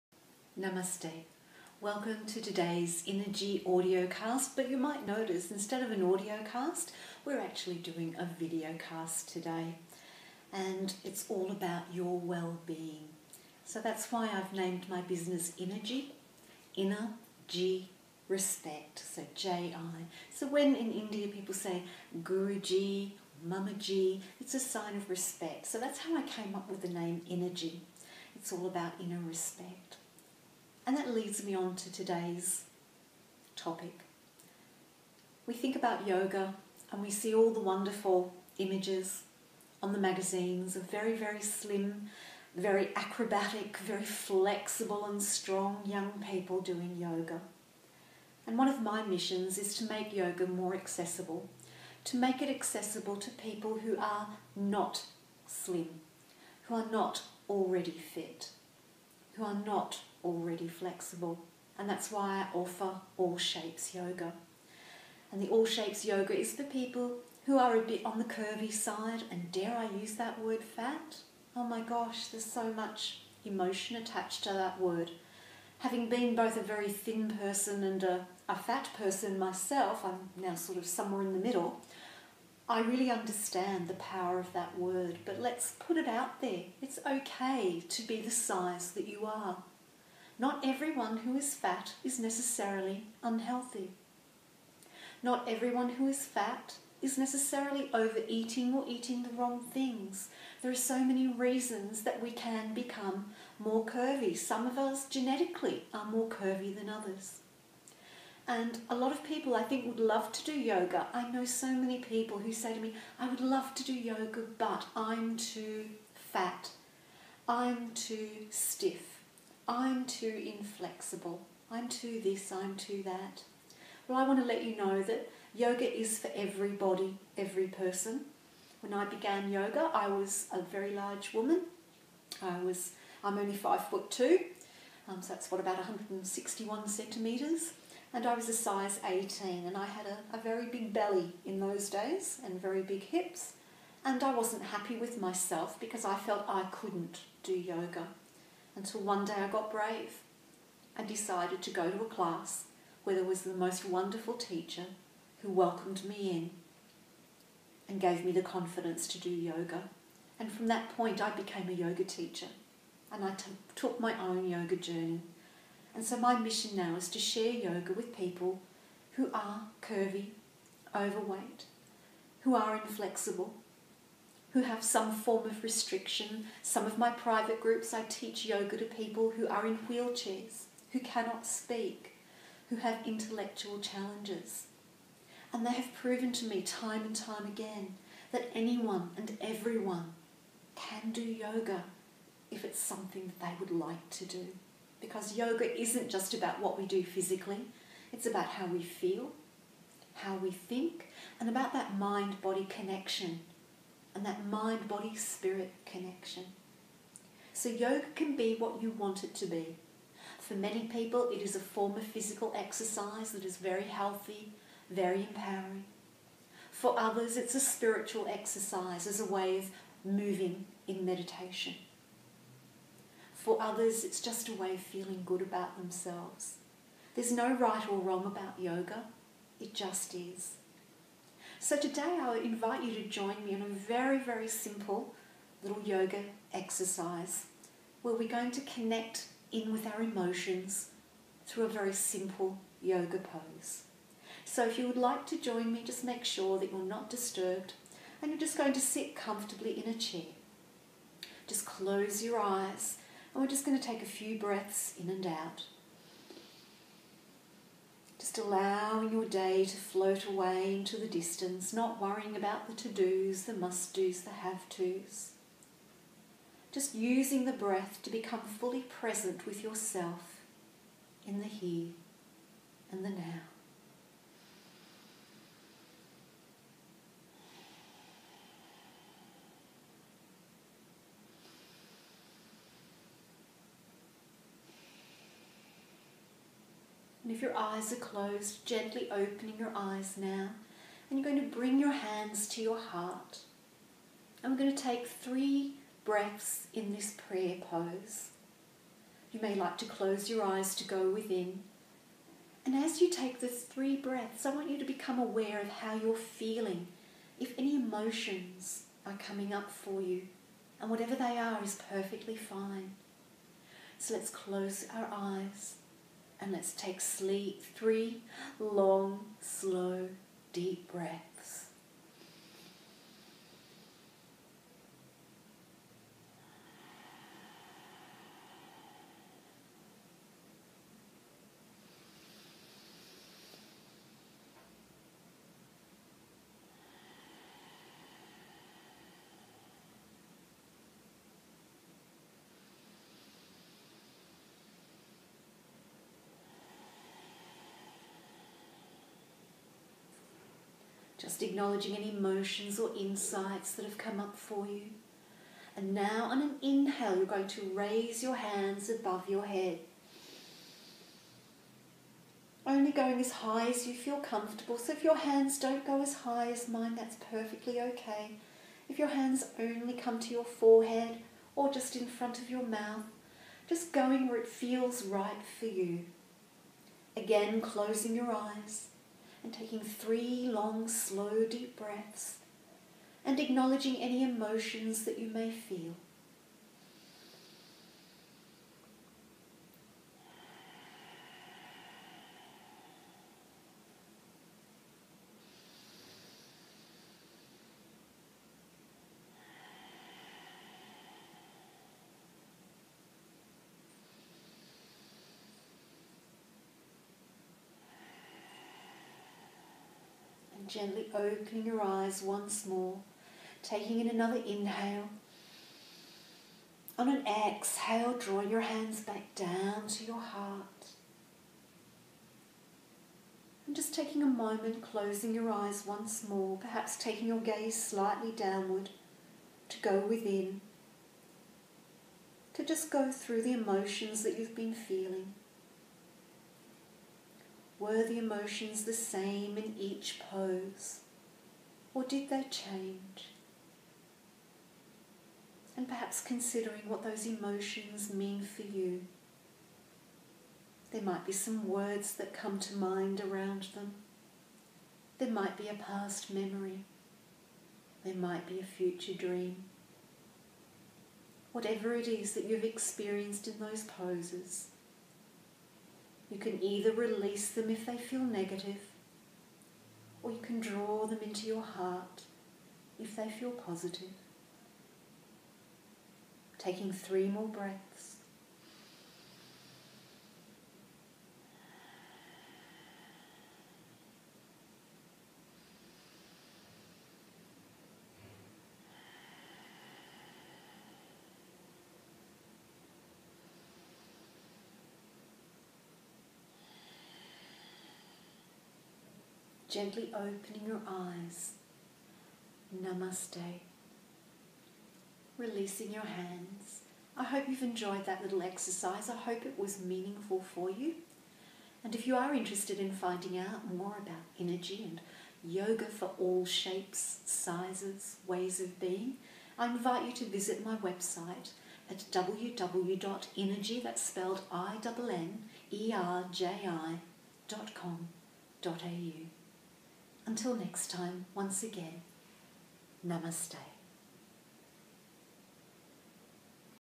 Body Positive Yoga by Innerji
Discover how yoga is for everyone regardless of size or shape and experience a yoga pose to connect with the emotions.

Innerji - yoga and well being - for every 'body' and every person